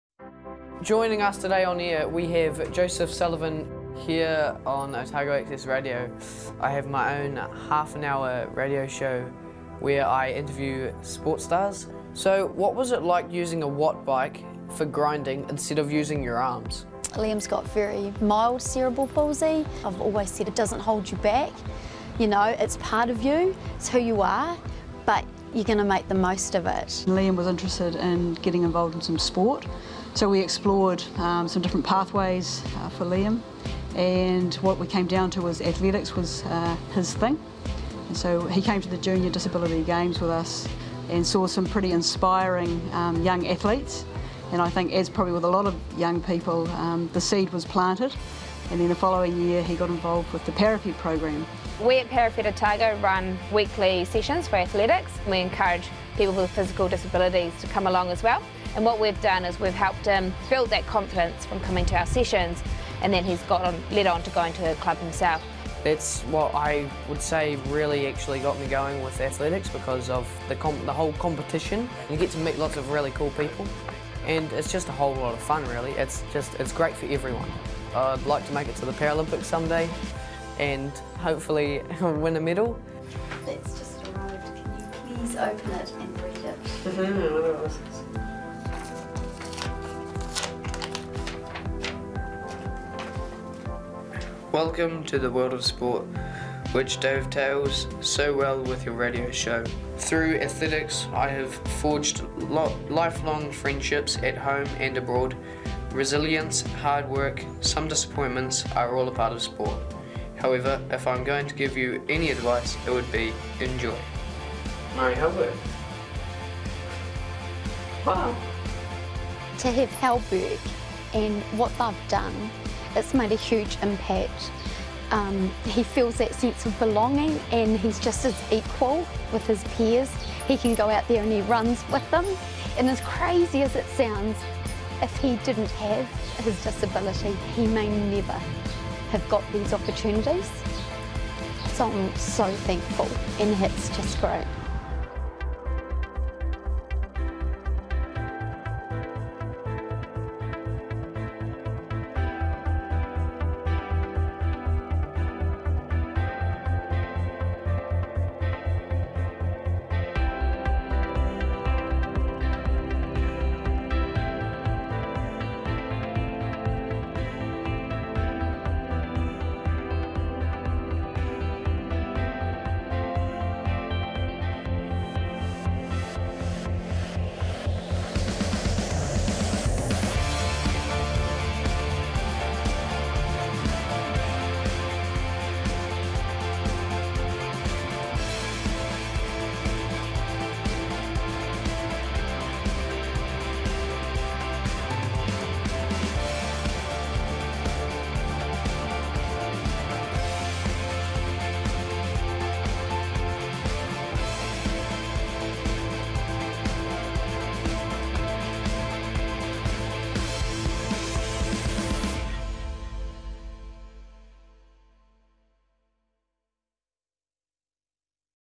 Liam Wilson
Liam Wilson is an aspiring athlete from Dunedin who found his passion for athletics at the Halberg Junior Disability Games. He also has his own radio sports show and in this video receives a very special letter from Sir Murray Halberg